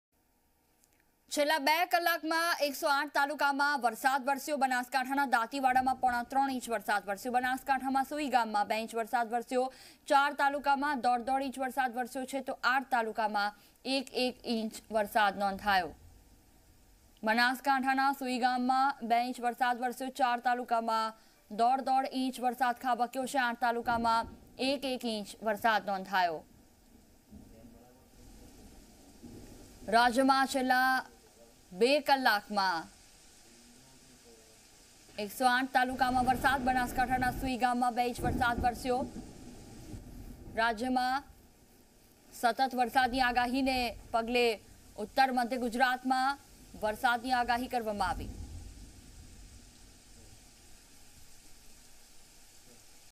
108 talukas of Gujarat record rainfall in the last 2 hours | Zee News
108 talukas of Gujarat record rainfall in the last 2 hours | Zee News

છેલ્લા 2 કલાકમાં 108 તાલુકામાં વરસાદ ---
બનાસકાંઠાના દાંતીવાડામાં પોણા ત્રણ ઈંચ વરસાદ ---
બનાસકાંઠાના સૂઈગામમાં 2 ઈંચ વરસાદ ---
4 તાલુકામાં દોઢ-દોઢ ઈંચ વરસાદ વરસ્યો ---
8 તાલુકામાં 1-1 ઈંચ વરસાદ નોંધાયો ---